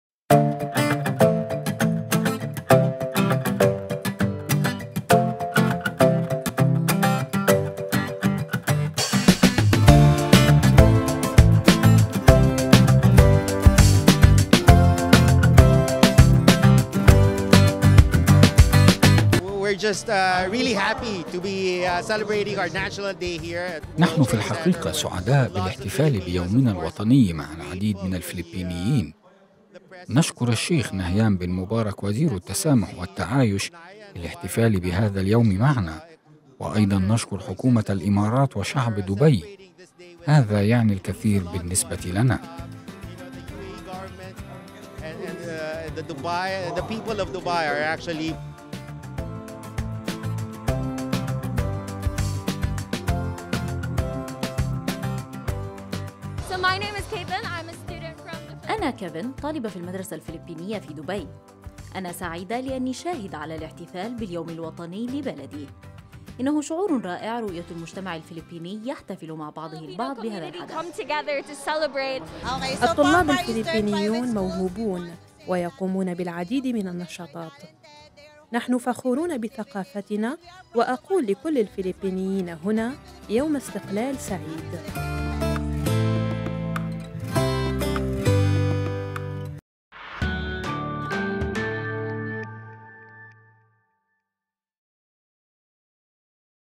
الجالية الفلبينية في الإمارات تحتفل بالذكرى الـ124لعيد الاستقلال
شهد الشيخ نهيان بن مبارك آل نهيان، وزير التسامح والتعايش في الإمارات لقاءً احتفالياً بمناسبة مرور 124 عاماً على استقلال جمهورية الفلبين